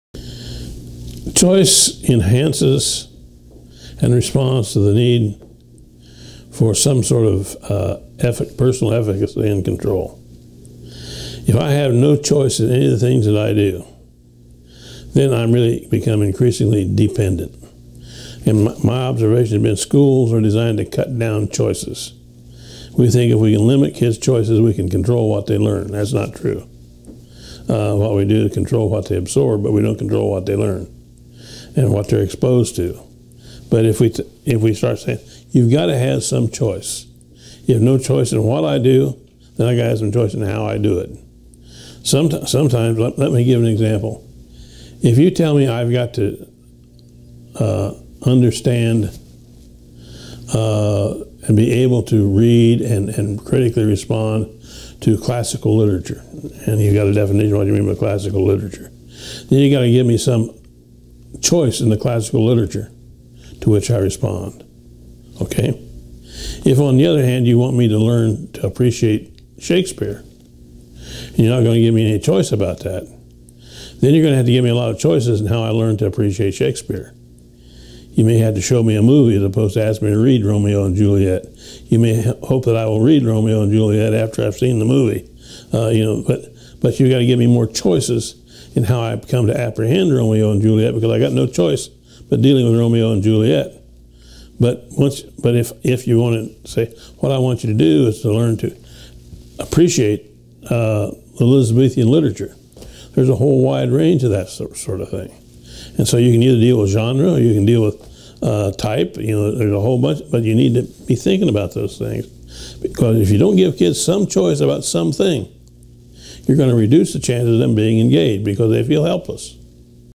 Choice mp4
Phil Schlechty explains what he means by the Design Quality of Choice.